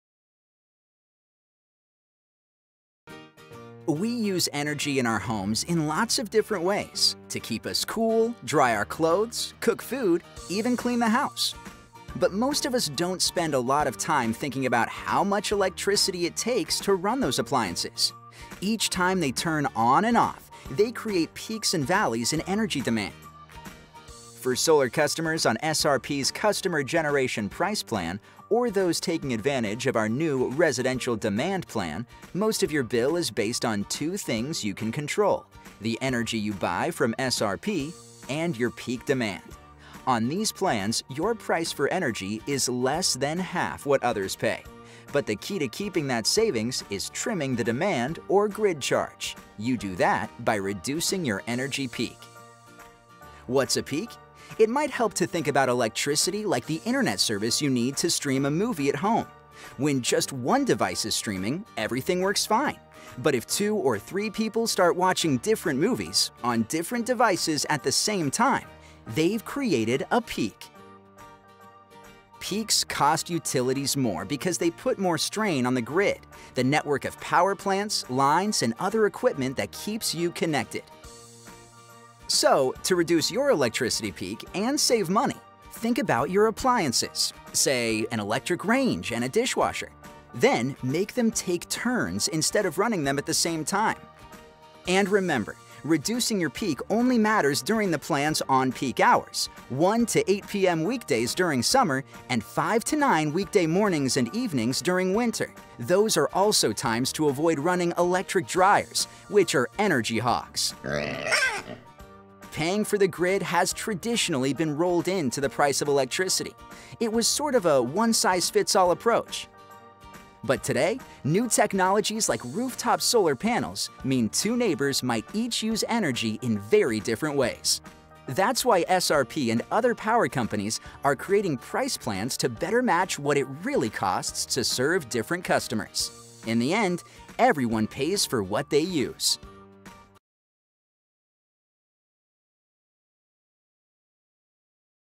SRP Energy Demand
SRP customer generation plan

for more info.

The Customer Generation Price Plan is the price plan for residential customers who choose to produce some of their own electricity using rooftop solar or other means.

The best way to save money on this plan is to:

Shift energy use from on-peak to off-peak hours. On-peak hours are weekdays from 5-9 a.m. and 5-9 p.m. November through April and weekdays from 1-8 p.m. May through October. All other times, including weekends and six holidays,* are off-peak.

Manage energy use in your home so major appliances that demand large amounts of electricity don't run at the same time during on-peak hours.

The Customer Generation Price Plan has the same on-peak hours as SRP's Time-of-Use (TOU) price plan, but is different in that it offers two ways to control your bill:

The energy charge on this plan is about half the rate of the TOU plan. It's also the lowest of any SRP residential price plan. Energy is the amount of power you consume over time.

There is a per-kilowatt demand charge, based upon your usage of the energy supplied by SRP. Demand is the amount of power your home needs at any given point in time. By limiting simultaneous use of appliances during on-peak hours, you can keep the demand charge lower. Your solar generation may help to offset some of your demand for energy from SRP during some on-peak hours, but because solar production can be affected by external conditions such as clouds, relying on it to lower your demand may be risky.

The monthly service charge, which helps cover the costs of grid access and maintenance, is $32.44 on this plan for most residential customers. It is $20 on TOU.